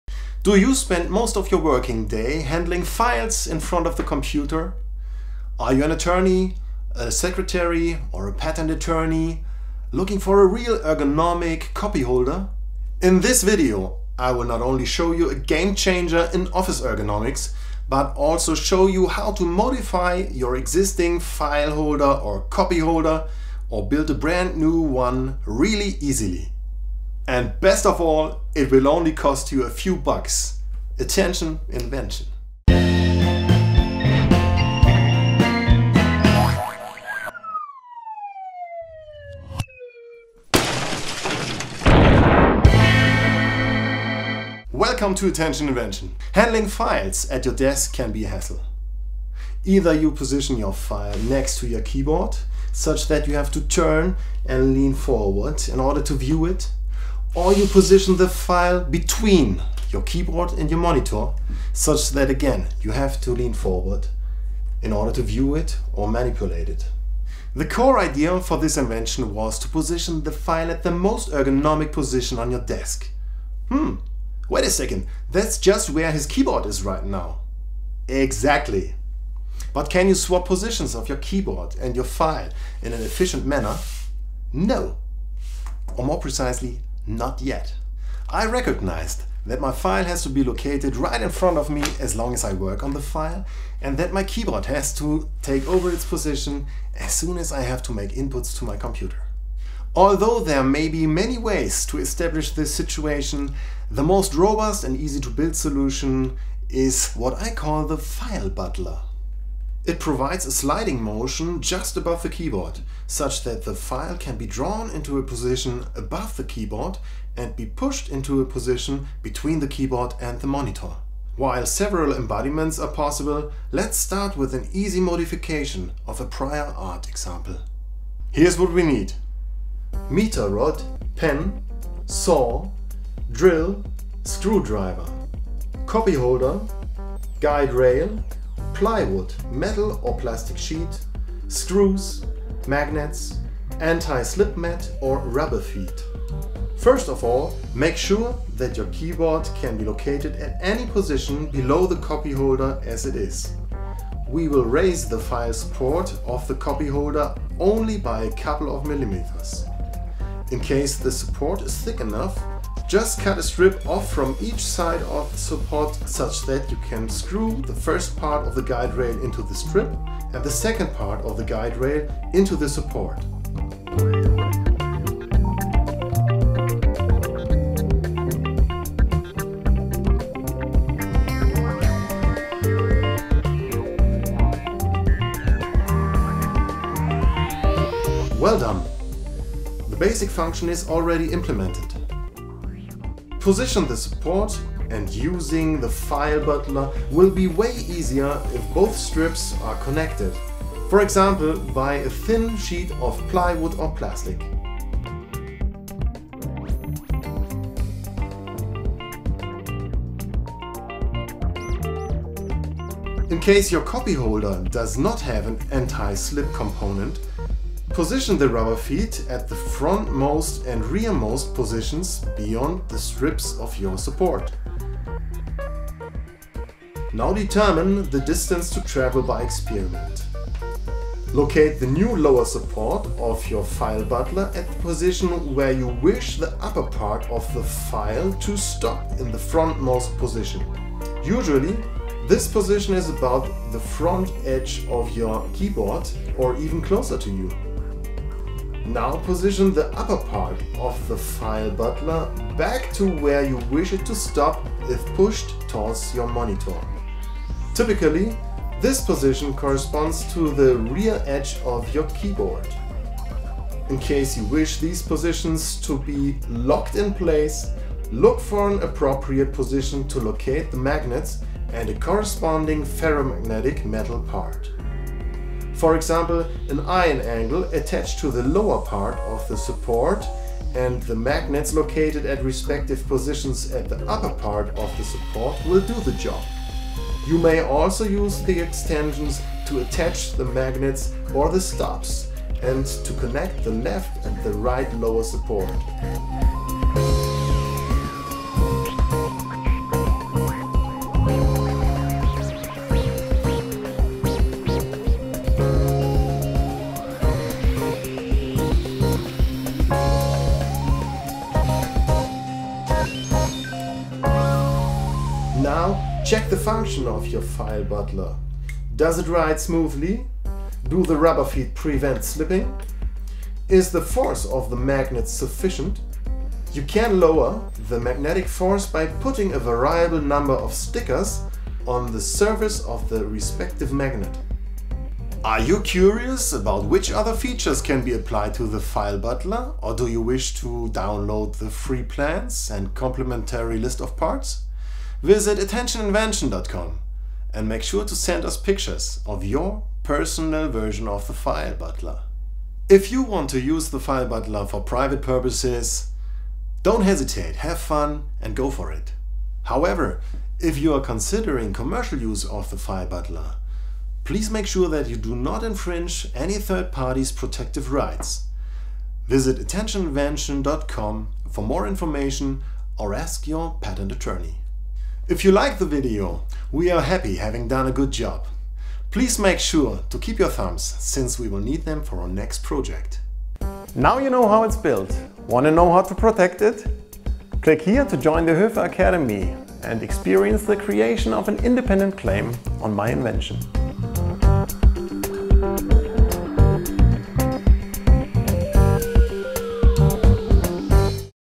Most Ergonomic Copy Holder EVER: We Build the Filebutler | DIY Tutorial (step-by-step instructions)
Your neck and shoulders hurt from hours in front of the computer? Maybe it's time for an ergonomic update of your desk?! In this video we build the most ergonomic copy holder (document holder) for your desktop.

Regardless of whether you are working at the office or at home, as soon as you are handling paperwork at your desk, a battle for the most ergonomic location is raging between your computer keyboard and the files and folders. Your ergonomic sweetspot right in front of you should always be owned by the item you are currently handling. This way you maintain the upright seating position and your shoulders stay where they belong. If we now manage to arrange the files at an ergonomic angle with resect to the desktop surface, you can effortless keep your head up and prevent a sore neck. But how to stow away your keyboard when handling the paper? And do this over and over again throughout the day?

The answer is the Filebutler. A horizontal guided motion arranges your inclined file support only centimetres above the keyboard or effortless positions it between the keyboard and the computer monitor if pushed forward. You have questions or already built your personal Filebutler?

Why wasn't this invented earlier you are asking? Good question! Only in 2020, the German Patent and Trademark Office (GPTO) granted a first patent (DE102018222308B4) on this solution which became an ergonomic game changer to attorneys, lawyers, secretarys, translators and many more.